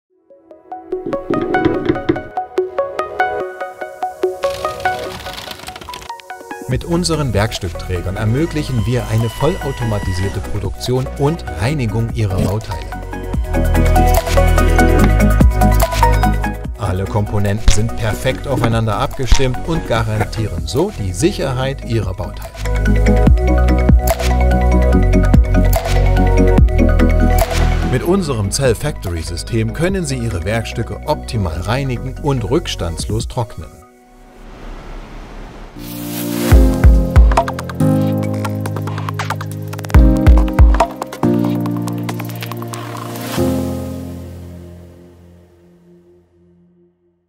Automation und Werkstückreinigung | ZELL Factory System | Nie wieder umchargieren!
Mit unserem neusten Werkstückträgersystem vereinen wir Transportsysteme der Produktion und Logistik. Umchargierungsprozesse können so deutlich reduziert werden. Das System eignet sich auch für die Bauteilreinigung, als Inlay für KLT oder Zuführung von Bauteilen in die Automation.